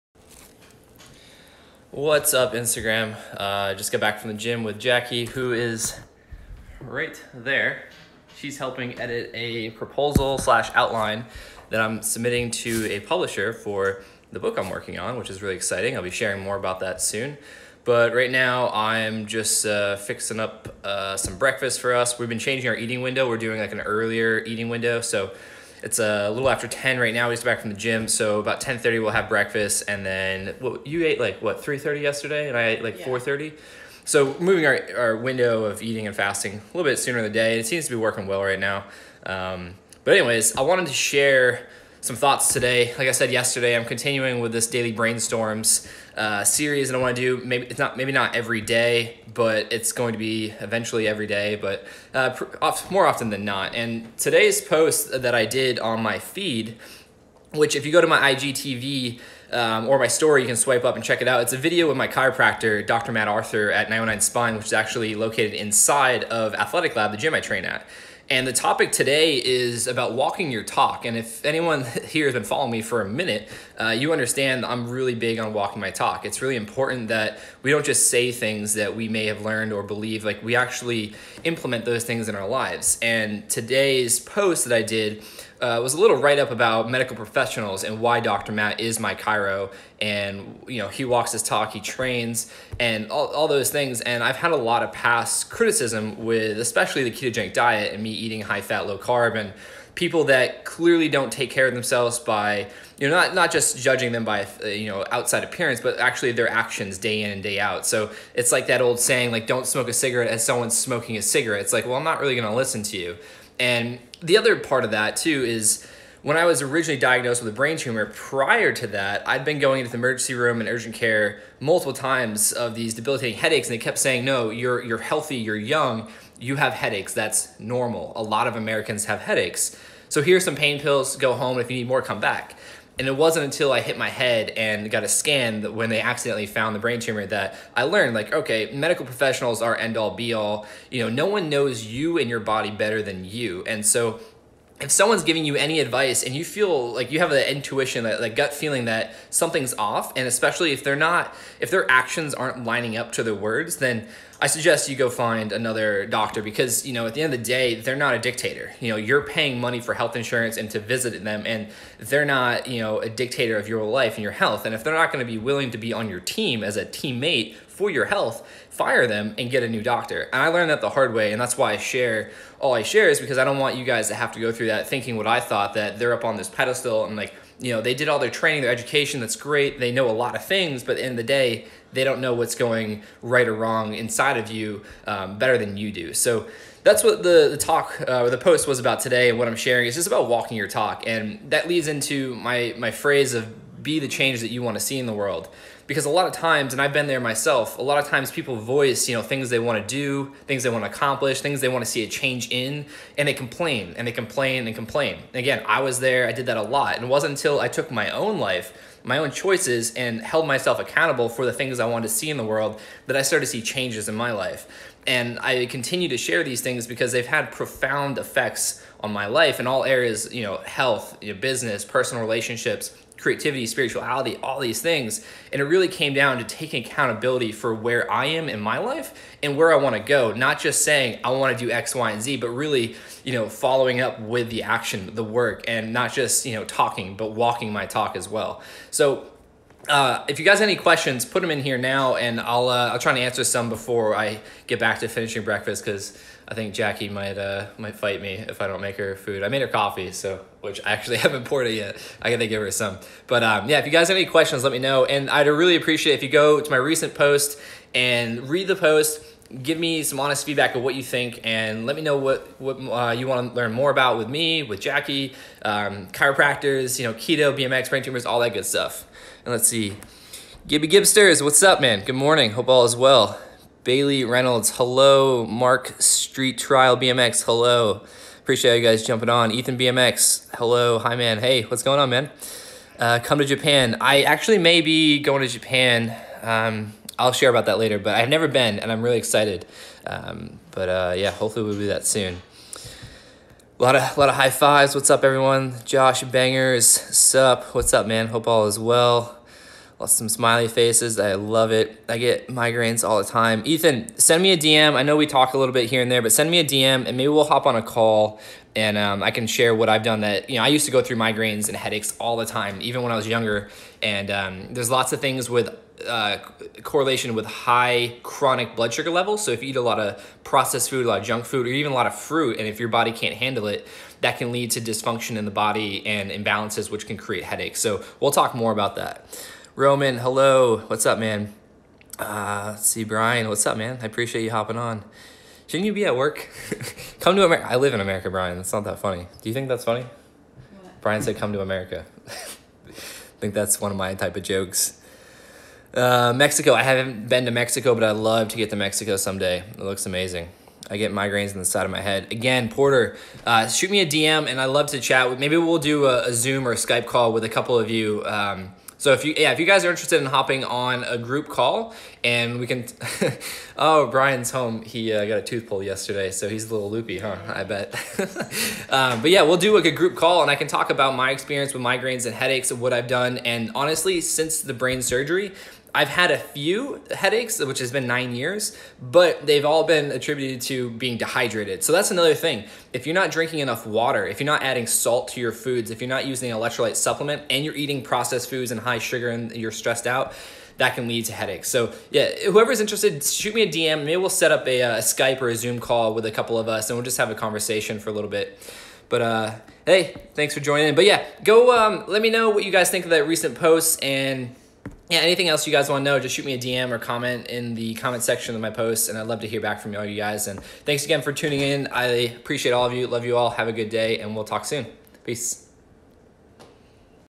Josh Perry | Instagram LIVE: Walk Your Talk
Instagram LIVE from Josh Perry talking about walking your talk and why it's so important to him. To see the original video, check this link to Daily Brainstorms episode #2

Let me know if you have any questions or concerns. I’d love to hear from ya.

Josh P. 💚🧠✌️

You can follow me on all social media at @JoshPerryBMX